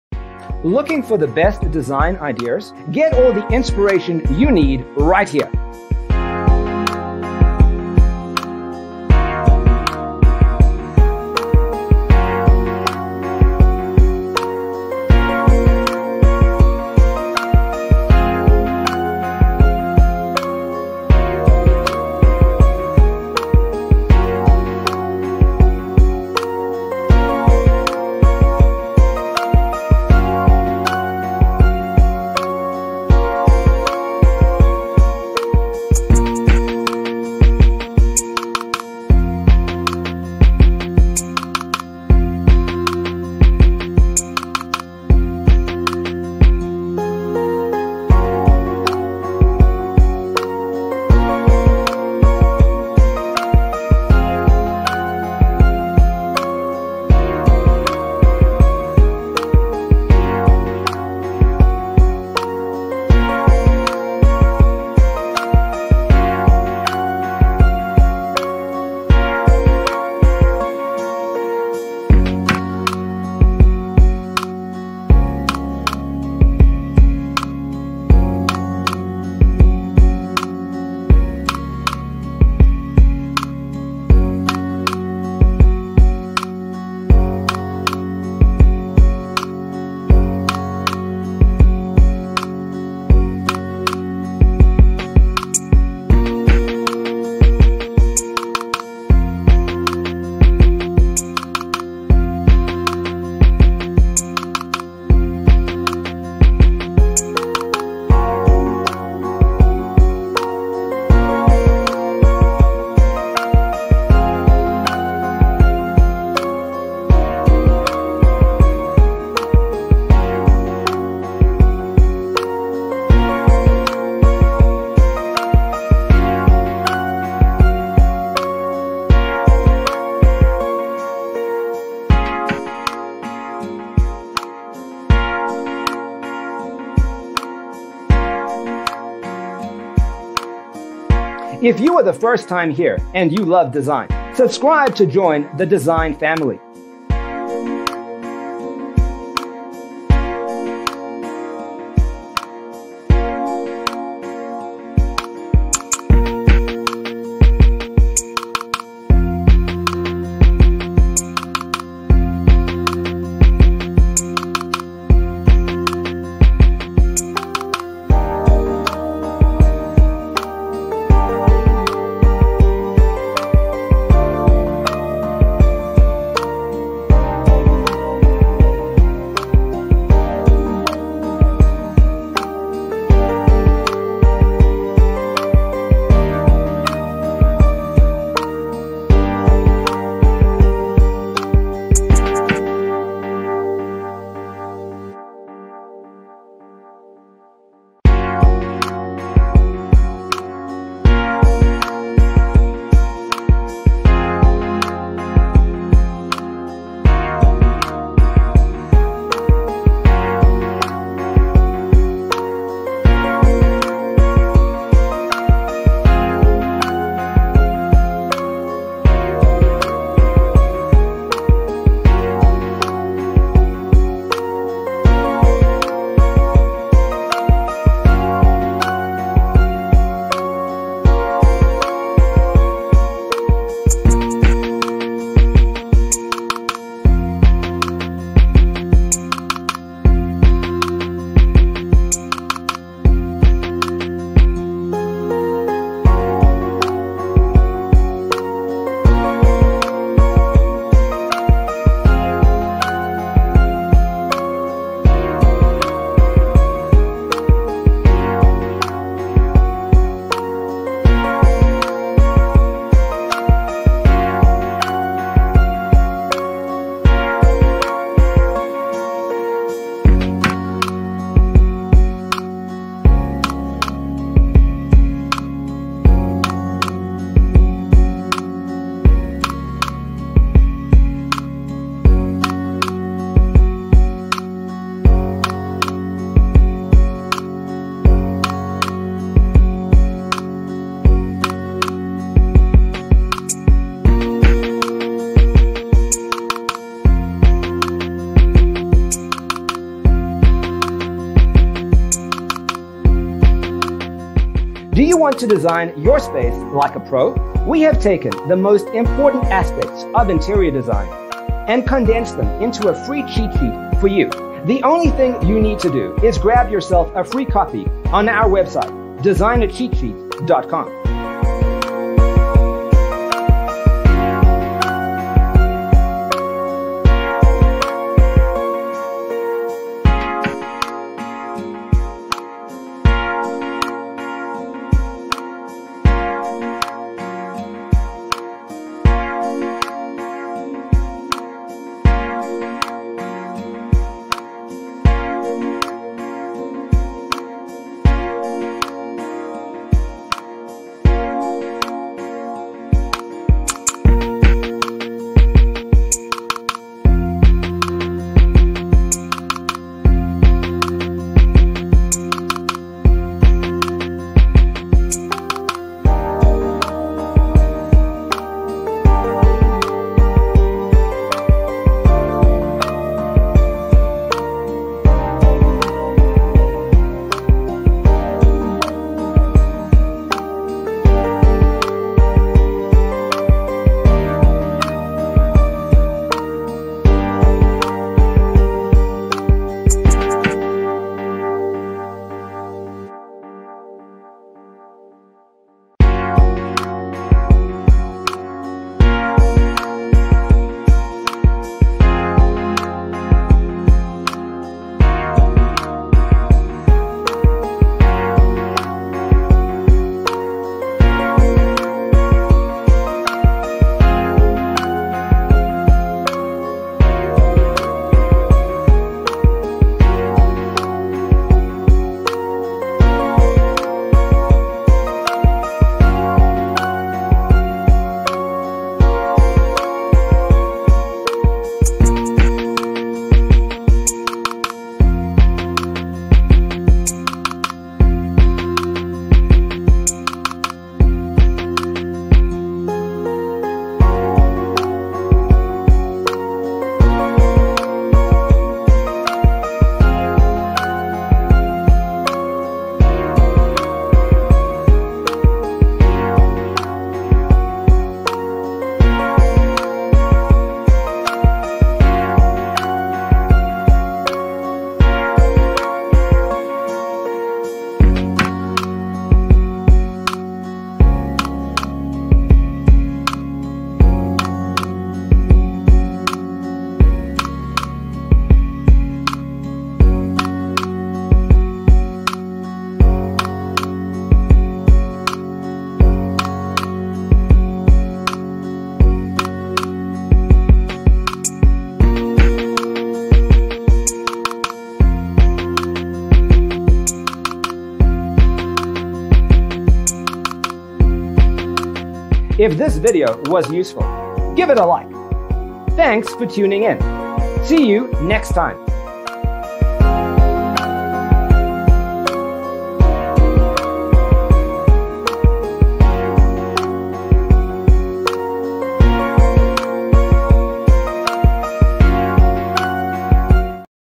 75 Huge Yellow Living Room Design Ideas You'll Love ☆
Browse Huge yellow living room decorating ideas and furniture layouts. Discover design inspiration from a variety of Huge yellow living rooms, including color, decor and storage options.

0:00:00 - Intro
0:00:12 - Huge Yellow Living Room Design Ideas You'll Love: Inspirations 1 to 25
0:03:03 - Huge Yellow Living Room Design Ideas You'll Love: Inspirations 26 to 50
0:06:07 - Huge Yellow Living Room Design Ideas You'll Love: Inspirations 51 to 75